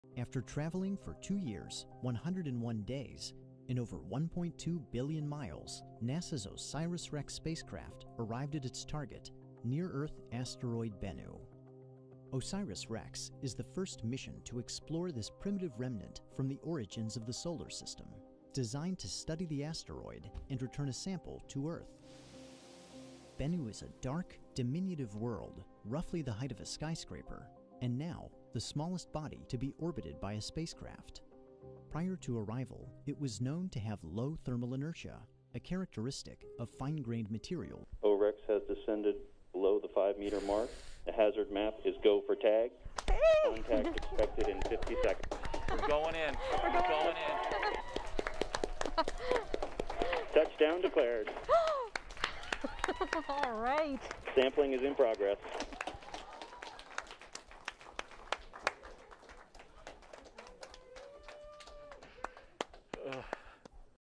NASA spacecraft touches surface of asteroid
(20 Oct 2020) A NASA spacecraft descended to an asteroid Tuesday and, dodging boulders the size of buildings, momentarily touched the surface to collect a handful of cosmic rubble for return to Earth.
It was a first for the United States _ only Japan has scored asteroid samples.
"Touchdown declared," a flight controller announced to cheers and applause. "Sampling is in progress."
Confirmation came from the Osiris-Rex spacecraft as it made contact with the surface of the asteroid Bennu more than 200 million miles away.
But it could be a week before scientists know how much, if much of anything, was grabbed and whether another try will be needed. If successful, Osiris-Rex will return the samples in 2023.
Osiris-Rex took 4 1/2 hours to make its way down from its tight orbit around Bennu, following commands sent well in advance by ground controllers near Denver.
Bennu's gravity was too low for the spacecraft to land _ the asteroid is just 1,670 feet (510 meters) across. As a result, it had to reach out with its 11-foot (3.4-meter) robot arm and attempt to grab at least 2 ounces (60 grams) of Bennu.